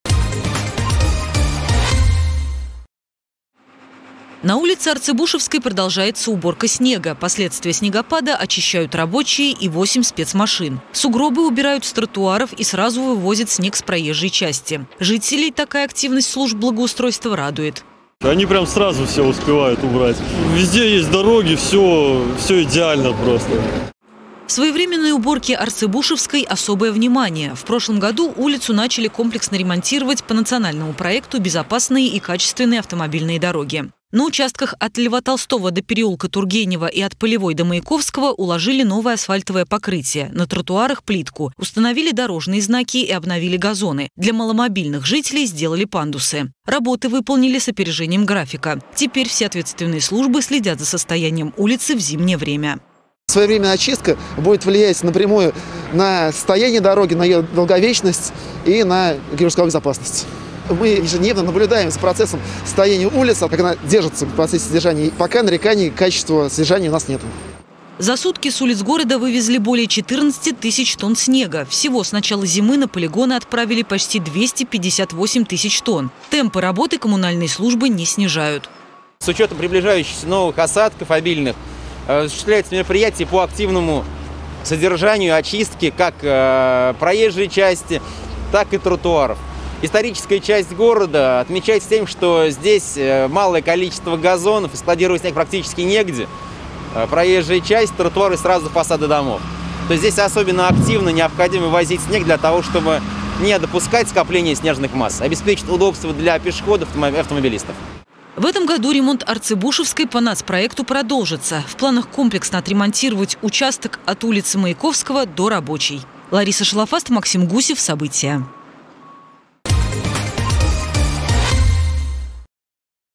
В Самаре продолжается уборка улиц
В Самаре продолжается уборка улиц. За сутки вывезли более 14 тысяч тонн снега. Порядок наводили и на Арцыбушевской, которую в прошлом году начали ремонтировать по национальному проекту "Безопасные и качественные автомобильные дороги". На уборке побывала наша съемочная группа.